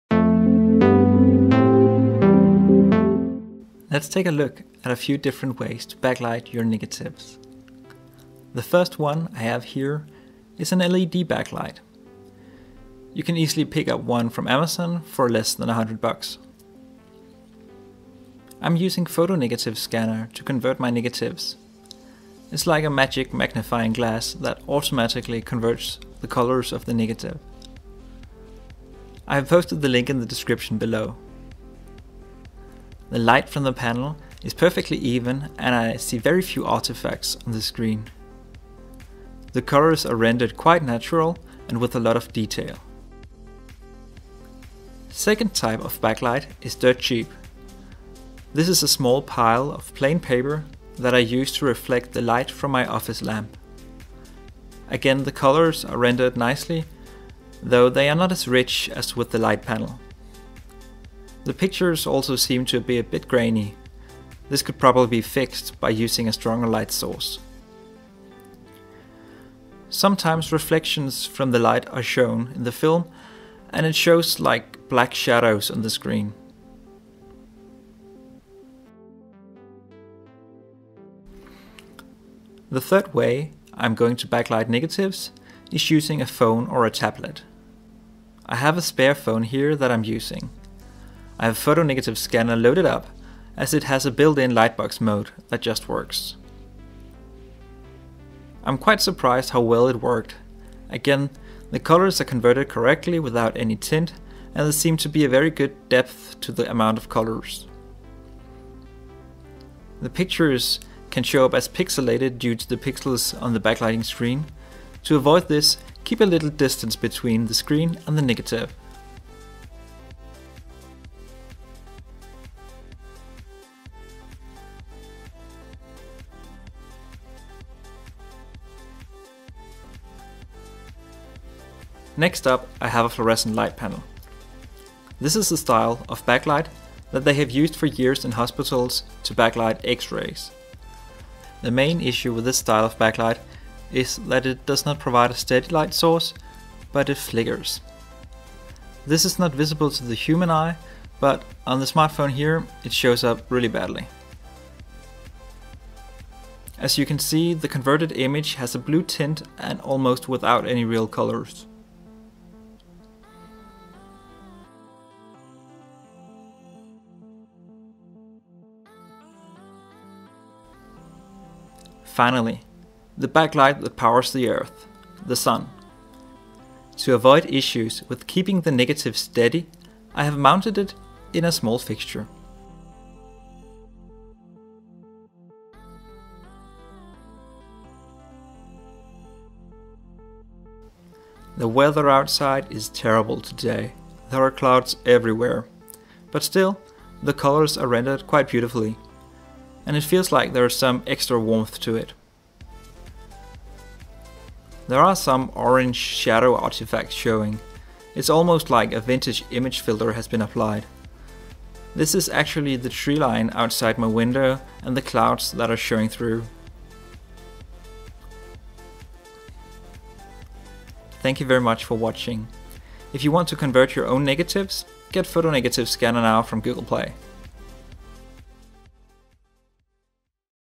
Five ways to backlight film negatives
In this video we demonstrate five different you can backlight photo negatives.
Methods, in order of appearance:
- LED Panel 00:03
- Plain paper 00:44
- Photo Negative Scanner light box mode  01:22
- Florescent light panel 02:11
- Sunlight 02:52

We are using the Photo Negative scanner app to view the negatives in real colors.

Summary:
The LED panel works the best.  It is easy to use and the result looks great.
Next up is the built in Light Box from the Photo Negative Scanner app.
Ac third place goes to "sunlight". Color rendition is great but clouds etc. can result in artistic artifacts.
Reflecting the light from a lamp using plain paper works fine but you will need a stronger light than I have here. You will also need to hold the film just right to avoid reflections.
The florescent light panel for xrays was not usable.